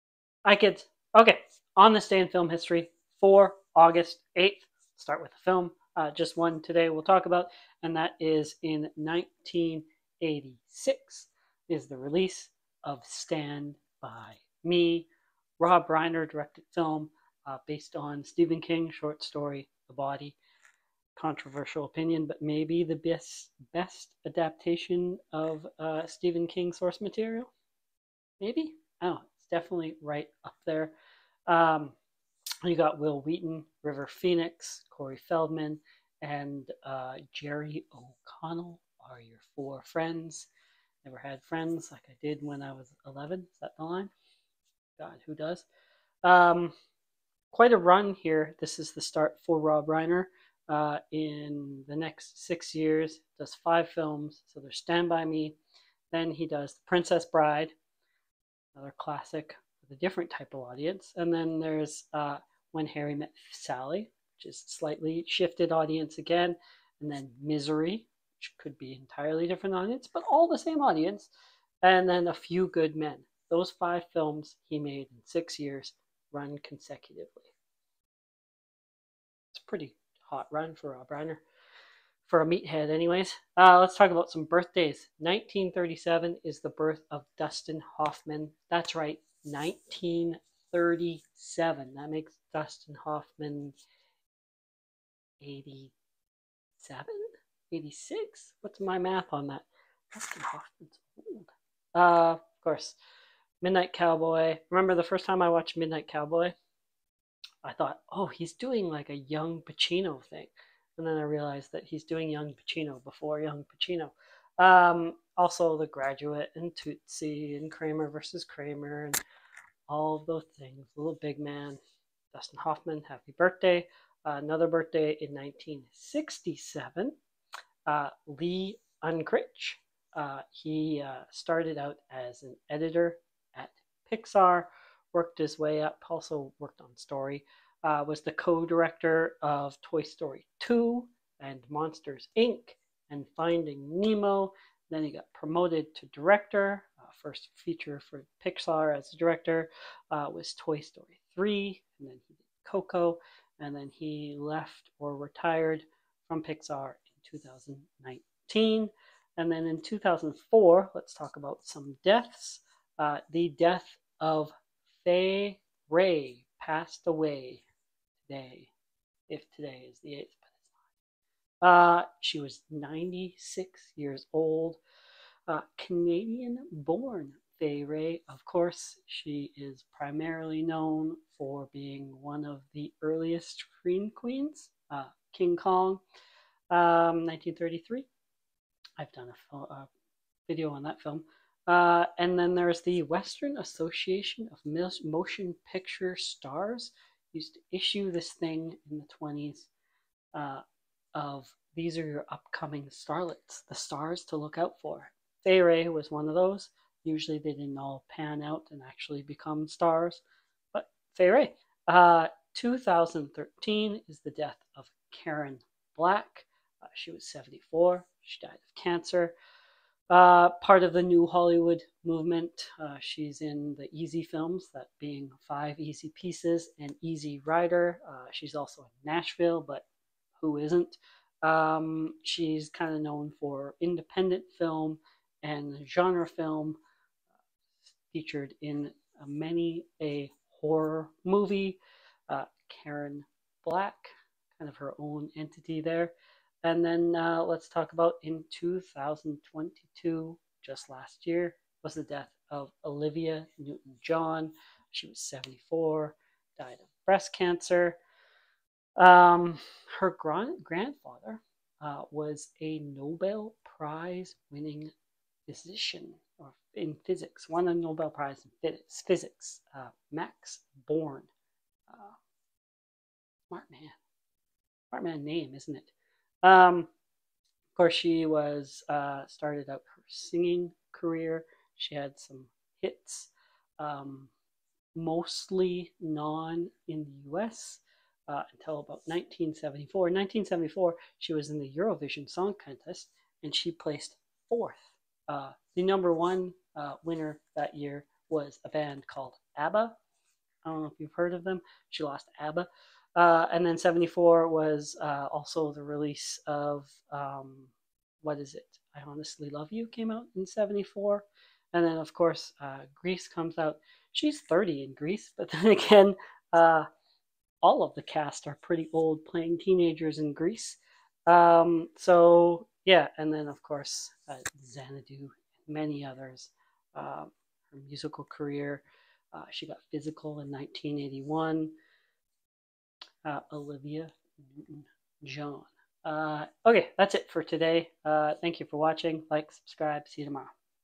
On this day in film history August 8th : Stand By Me, Dustin Hoffman and Olivia Newton John
A look at film history for August 8th